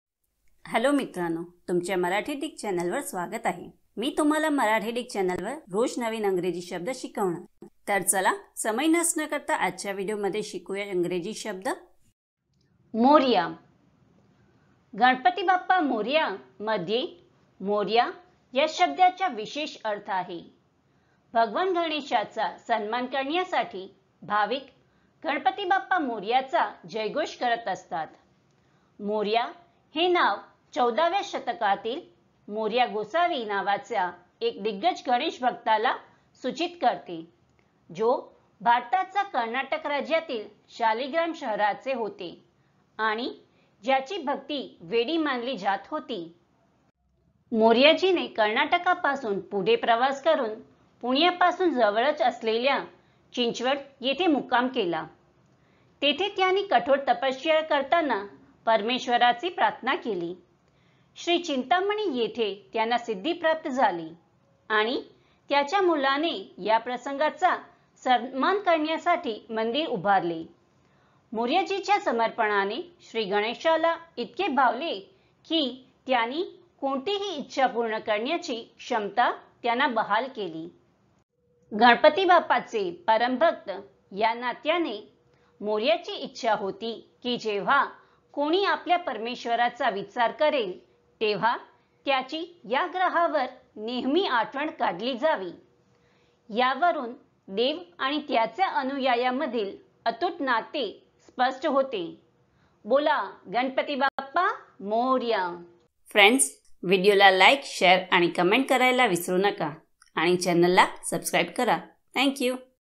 Morya Meaning in Marathi | Morya म्हणजे काय | Morya in Marathi Dictionary
या व्हिडिओमध्ये तुम्हाला मराठीत Morya चा अर्थ समजेल आणि Morya चा उच्चारही शिकाल. याचा अर्थ तुम्ही Morya चा अर्थ जाणून घ्याल तसेच Morya कसे उच्चारता येईल, Morya म्हणण्याचा योग्य मार्ग कोणता आहे.

तर Morya शब्द फक्त एका मिनिटात शिका.

उदाहरणांसह Morya समजून घेऊ.